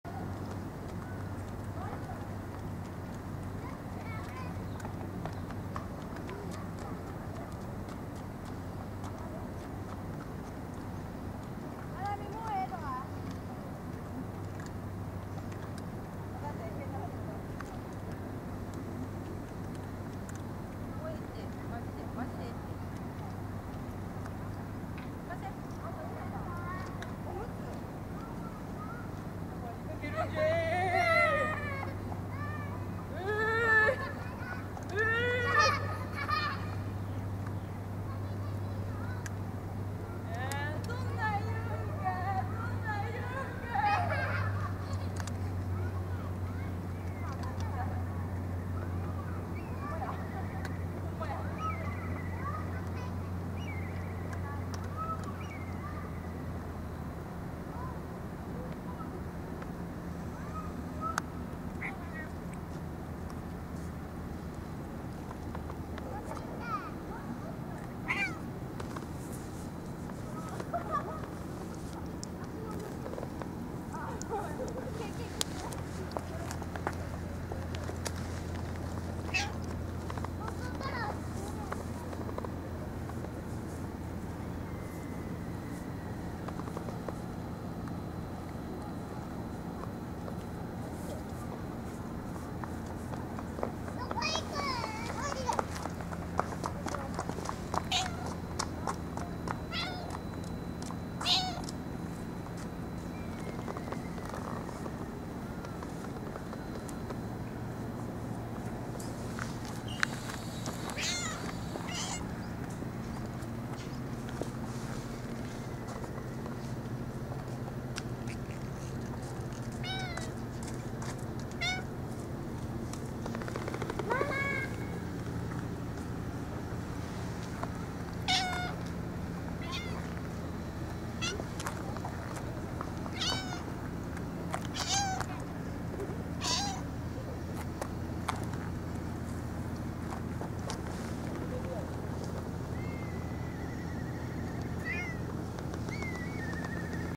大蔵海岸地域猫2015 7 18b
サビ子です。彼女は　パウチ
缶詰類は　食べないので
カリカリが主食です。